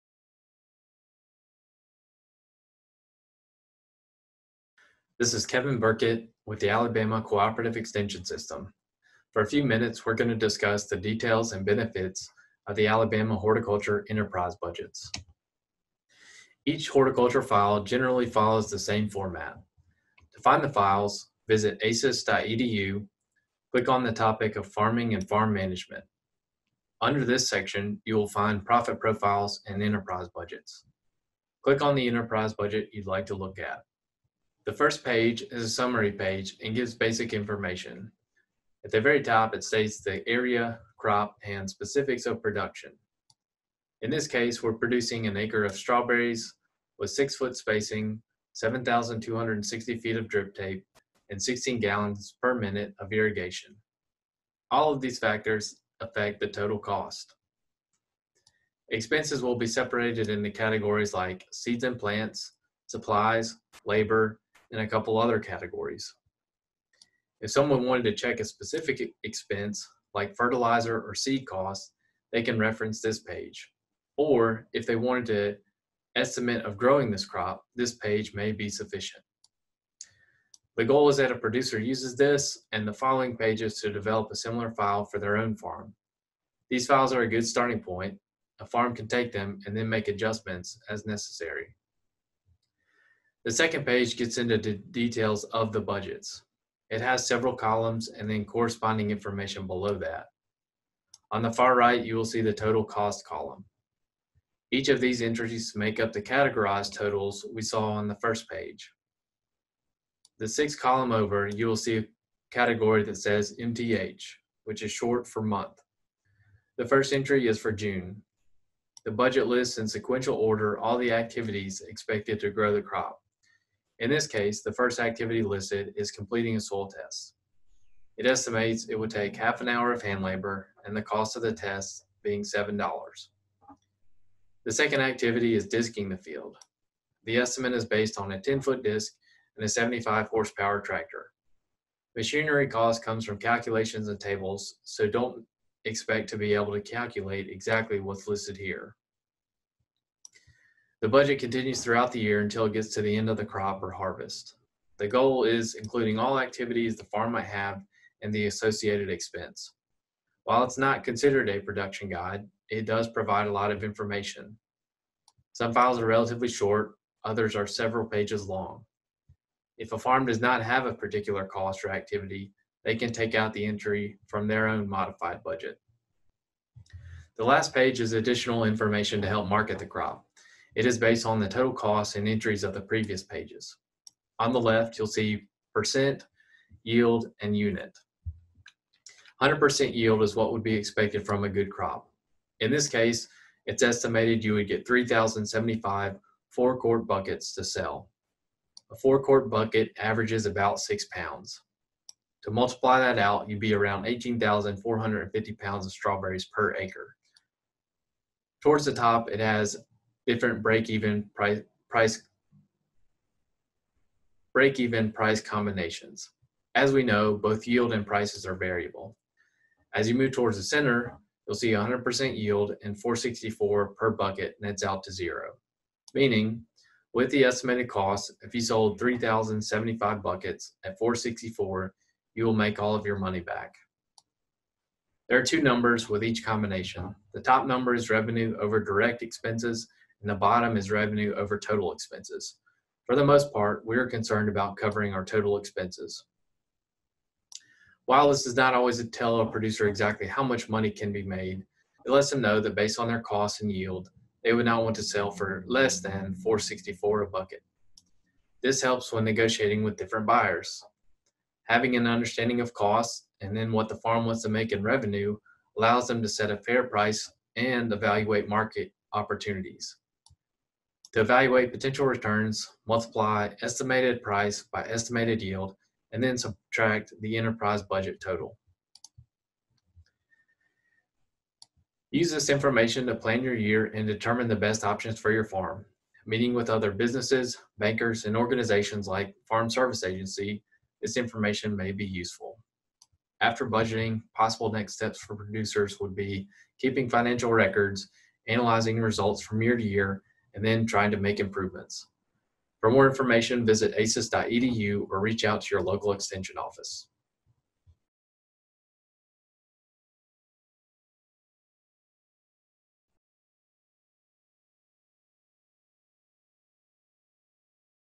Kevin Burkett: Horticulture Budget Explanation
A discussion of the details and benefits of the Alabama Horticulture Enterprise budgets.